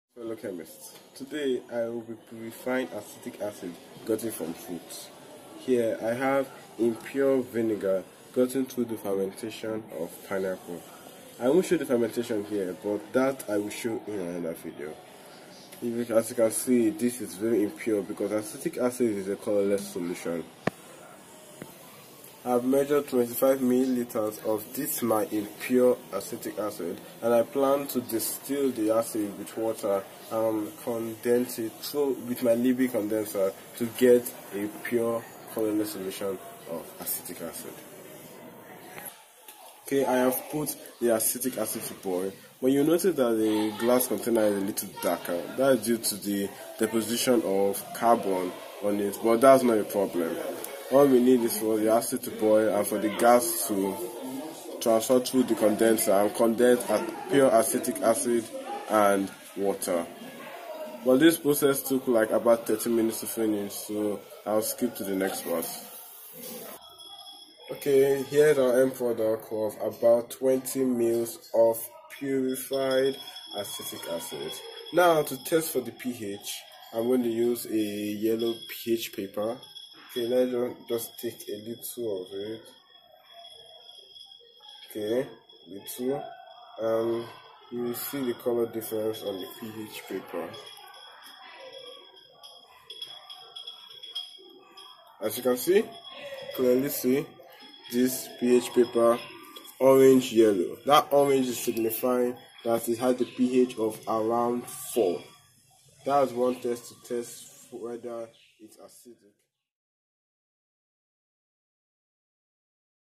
Make 100% PURE Acetic Acid from Vinegar at Home
This video showcases how you can extract acetic acid from impure vinegar obtained from fermentation
The fermentation process has been showcased in another video so click the link to know everything on fermentation

     ▶️ To obtain acetic acid from vinegar, follow the outlined steps
Step 1. Put your homemade vinegar in a suitable glass vessel
Step 2. Transfer the vessel to a hotplate/Bunsen burner and with the help of a condenser, distill the pure acetic acid into another vessel

       You can test your pure acid by doing a litmus test. It would show an orange color for a universal indicator and would turn a blue litmus red.